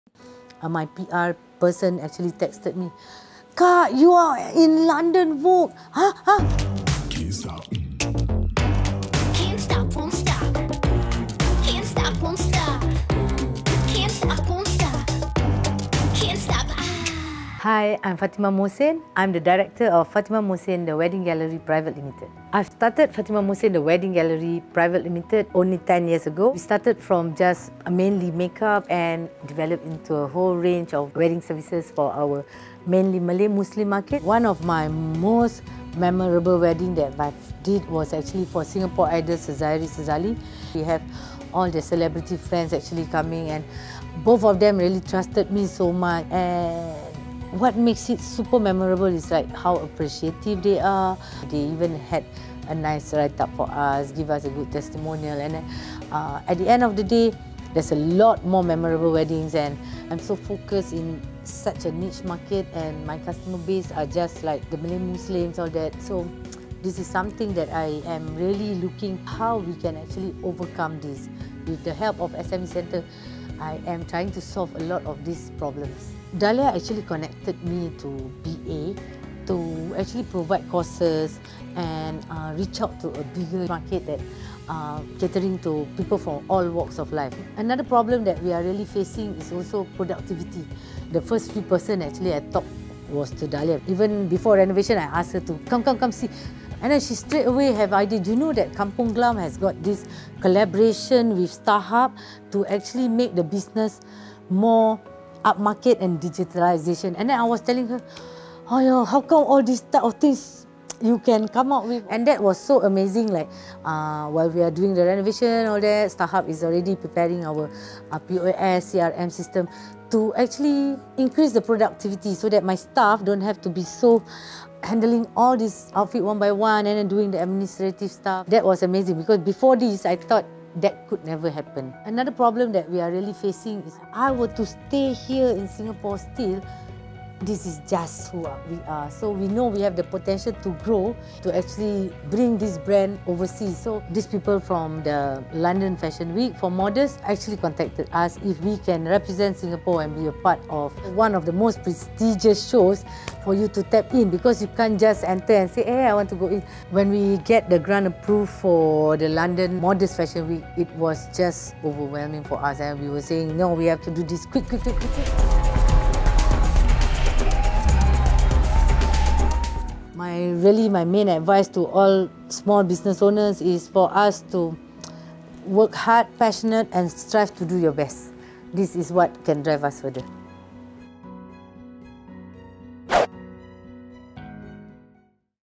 Growing With: Fatimah Mohsin The Wedding Gallery
Grow your business with the SME Centres. Local SME Fatimah Mohsin The Wedding Gallery organised Sezari Sazalli's wedding and made it to the world stage at London Modest Fashion Week 2018.

For more ways to improve your business capabilities and go overseas,